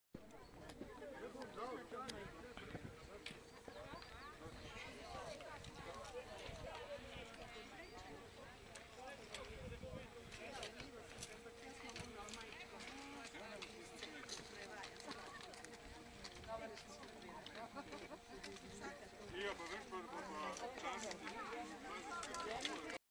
Vojak - najvišji vrh Učke (1401m) - 10. 4. 2010
Sobotni izlet Planinskega društva Lisca Sevnica